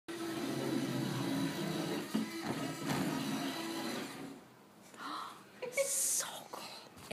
Kinder robotics : angelfish
Amazing stuff from 5-6 yr old students